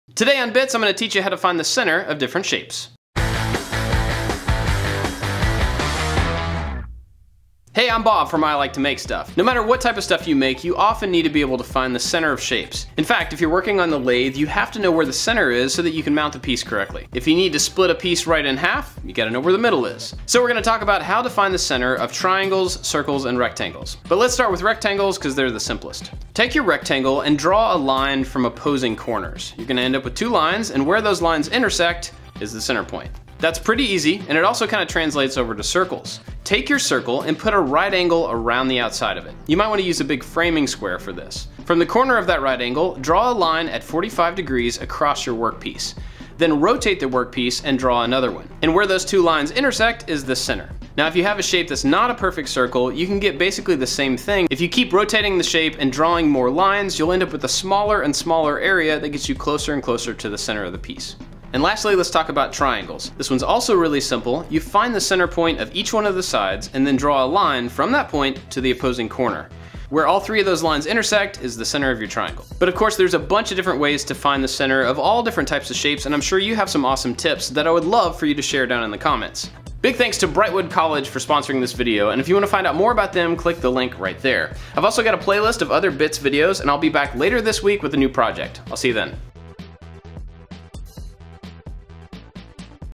Finding Center // Bits | I Like To Make Stuff
Some times you need to find the center of different shapes. In this Bits video I'll show you a few ways to do it.